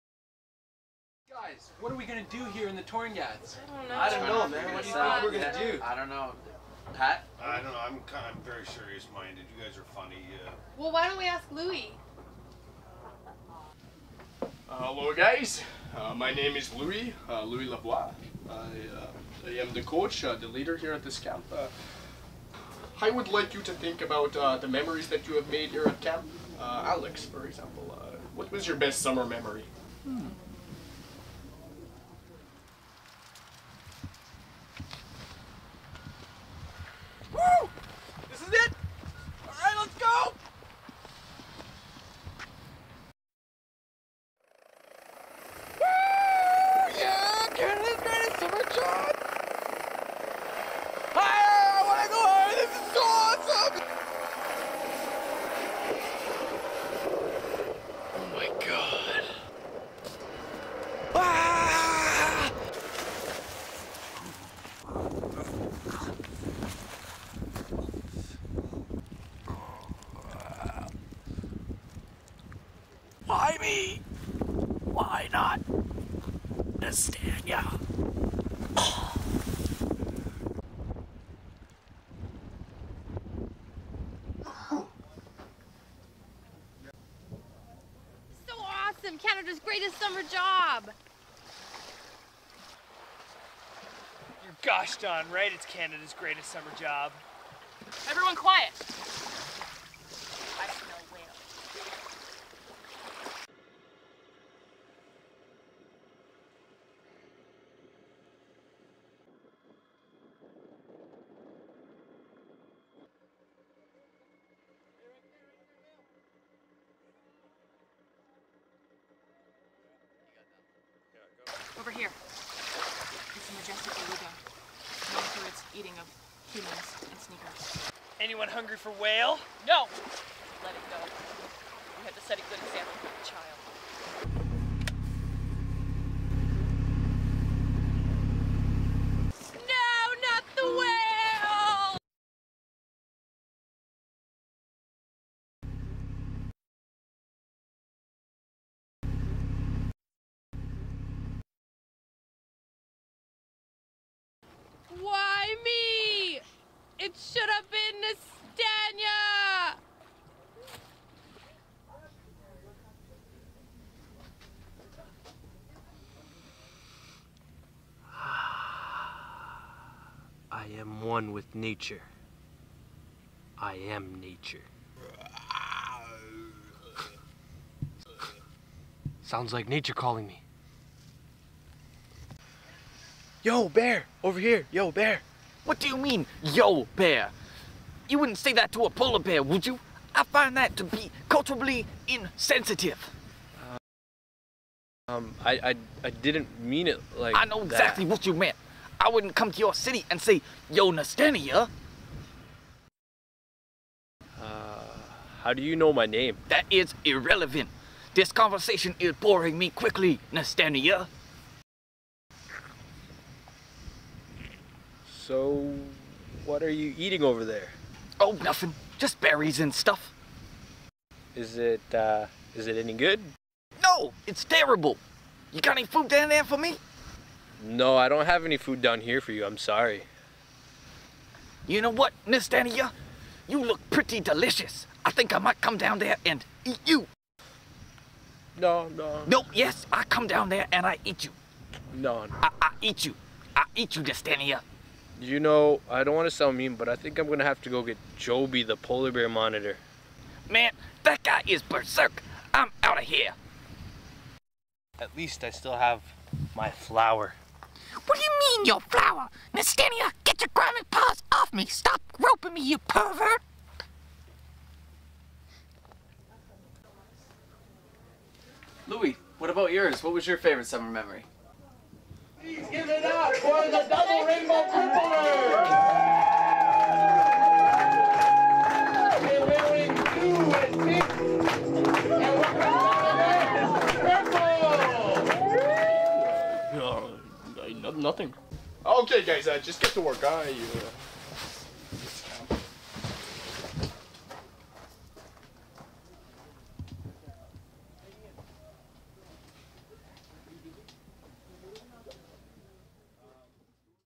Torngats Video
Crazy video from the Canada's greatest summer job trip to Torngat Mountains National Park